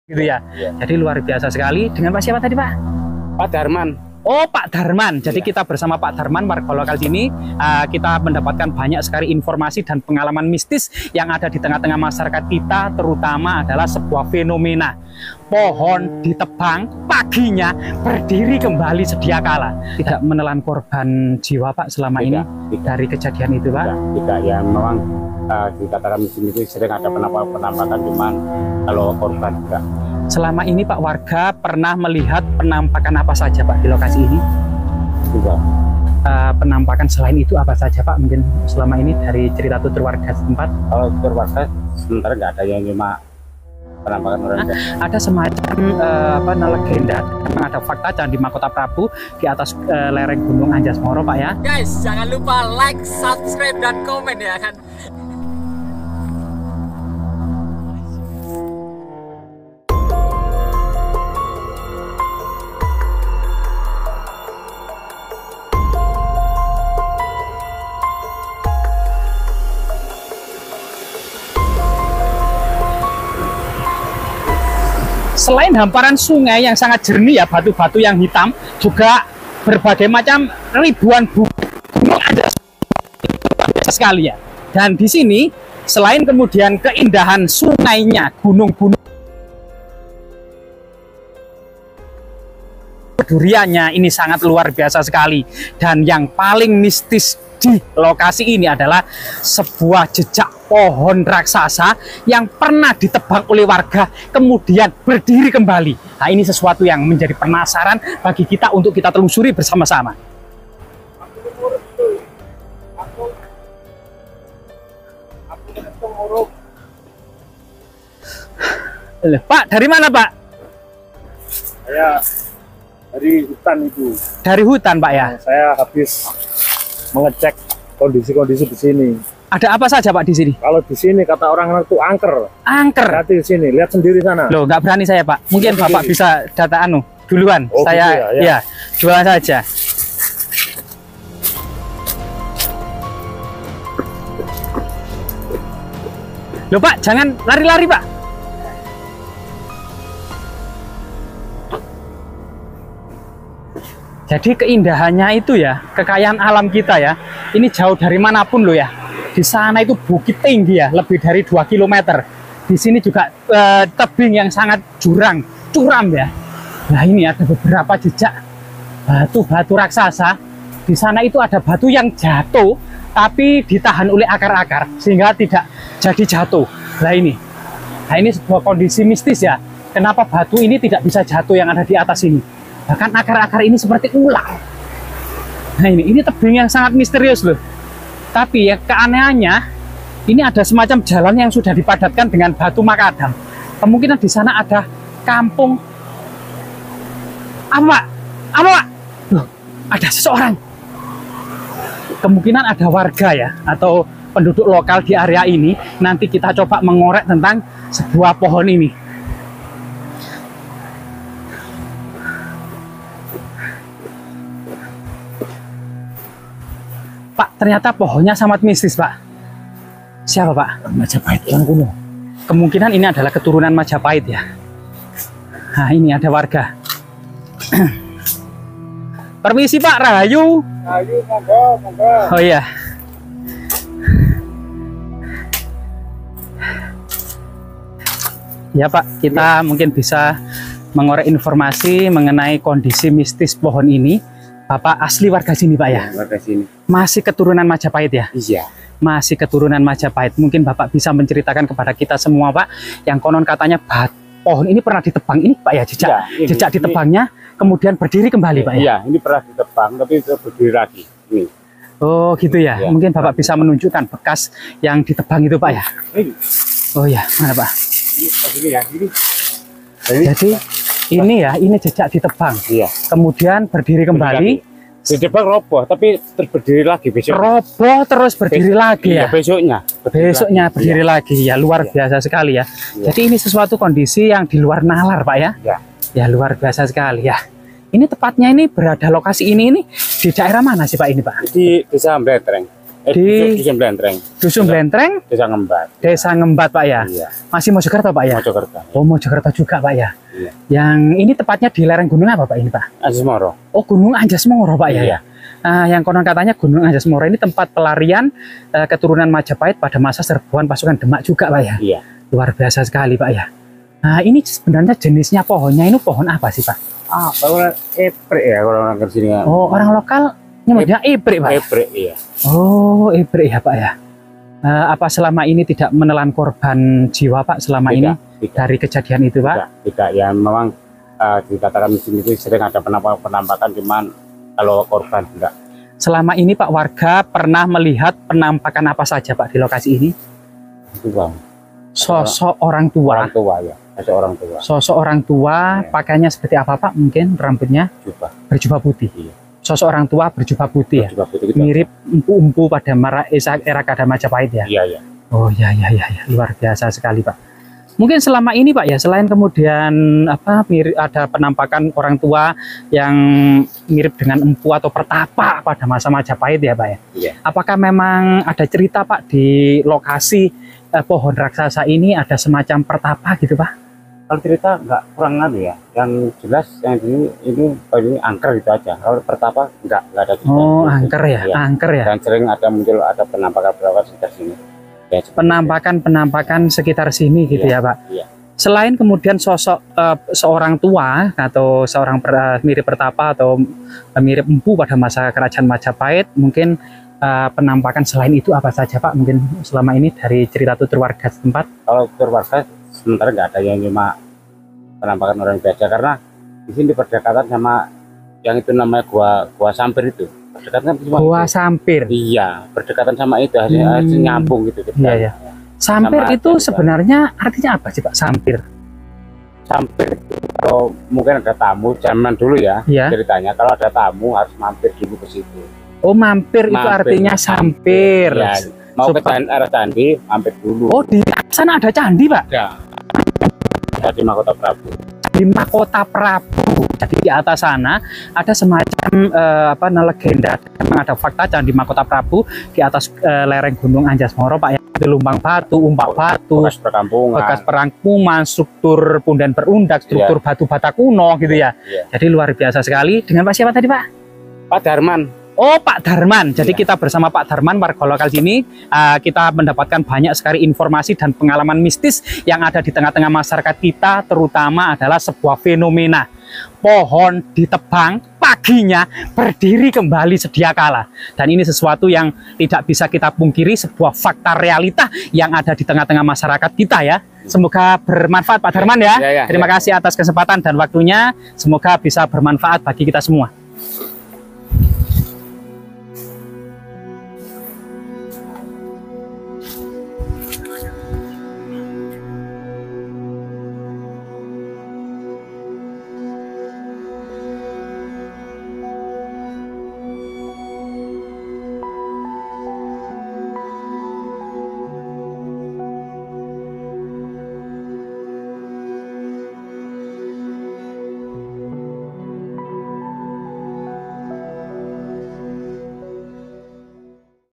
WARGA GEMPAR !! DITEMUKAN POHON DITEBANG BERDIRI LAGI DI GUNUNG ANJASMORO JAWA TIMUR ?!
Terutama jenis pohon raksasa berusia ratusan tahun baik di hutan belantara maupun di lokasi yang dikeramatkan warga setempat.
Biasanya pohon beringin raksasa dengan akar-akar yang sangat besar termasuk percabangannya.
Sebagian masyarakat tentu menghubungkannya dengan hal mistis. Namun sebagian lainnya melihat dalam perspektif hukum alam, keseimbangan berat dan gaya gravitasi.
Kali ini kita mewawancarai salah satu warga desa setempat bernama pak Darman yang diduga melihat kejadian tersebut, ulasannya bisa disaksikan di video selengkapnya di YouTube Majapahit Study Club.
Pohon beringin raksasa ini berbalut akar-akarnya sehingga terlihat sangat tambun dan tentunya sangat berat.
Jika pohon ini ditebang akan roboh meskipun belum sampai putus karena batang dan cabangnya sangat berat.
Ketika cabang-cabangnya dipotong dimungkinkan pohon ini bisa berdiri lagi karena berat dari cabang-cabangnya hilang.
Pohon beringin juga merupakan pohon yang sangat mudah sekali bertahan hidup meskipun hanya sebagian kecil akarnya yang tersisa.
Salah satu pohon beringin indah dan rindang ini terletak di pinggir sungai Galuh di lereng gunung Anjasmoro.
Nama sungai Galuh ini dimungkinkan diambil dari cerita pernikahan Pangeran Panji Asmarabangun dari Jenggala dengan putri Galuh Candrakirana dari Panjalu.
Sedangkan nama Anjasmoro dimungkinkan diambil dari nama putri Patih Loh Gender dalam cerita serat Damar Wulan.
Selain itu gunung Anjasmoro yang satu kluster dengan gunung Welirang, Arjuno dan Penanggungan (Pawitra) dalam kajian artefaknya juga diduga tempat pelarian keturunan Majapahit selama masa-masa ekspansi Kesultanan Demak.
Terletak di selatan dusun Blentreng, desa Ngembat, Gondang, Mojokerto, Jawa Timur.